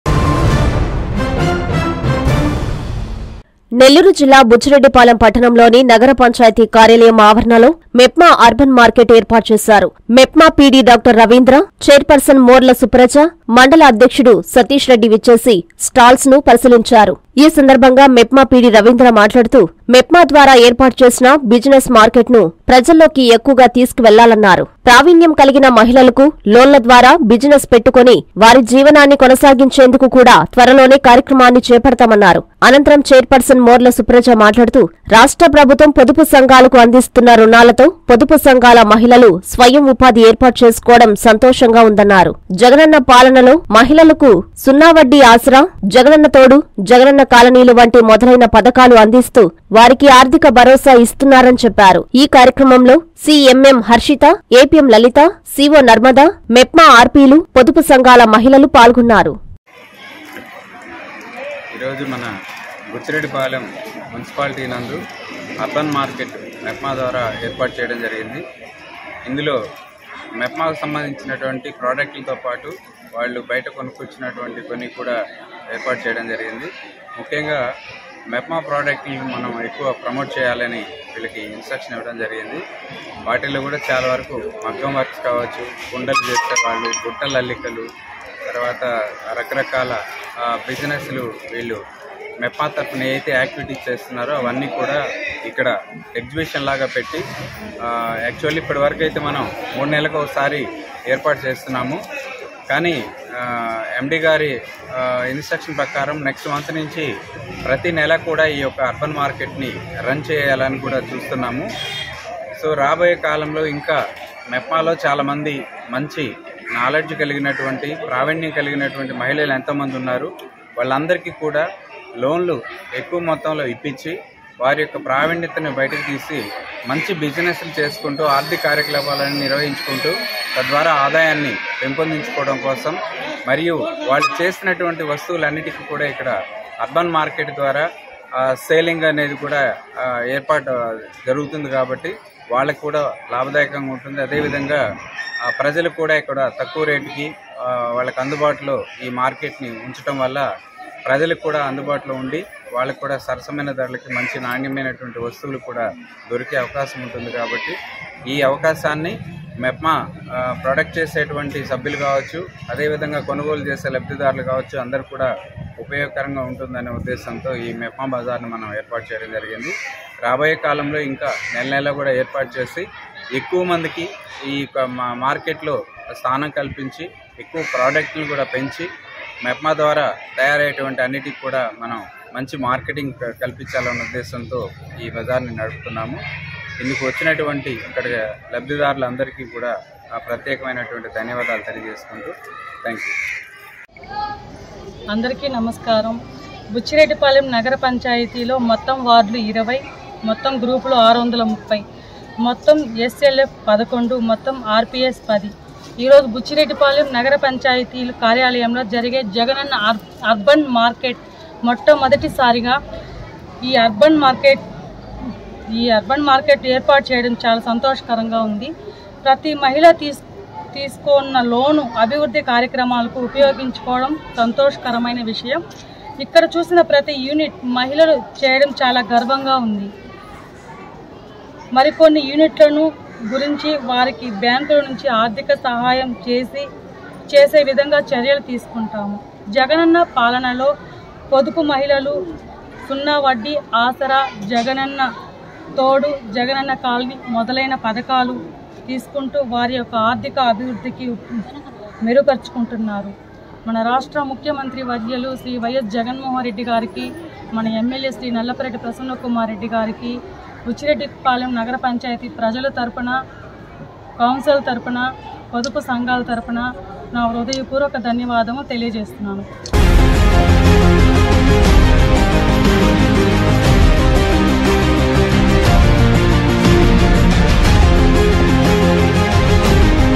బుచ్చిలో మెప్మా అర్బన్ మార్కెట్ ఏర్పాటు | MEPMA Urban Market | Buchireddypalem
- ప్రావీణ్యం క‌లిగిన మ‌హిళ‌ల‌ను ప్రోత్స‌హించేందుకు లోన్లు మంజూరు
- స్టాల్స్‌ను ప‌రిశీలించిన మెప్మా పీడి డాక్టర్ రవీంద్ర, చైర్ పర్సన్ మోర్ల సుప్రజ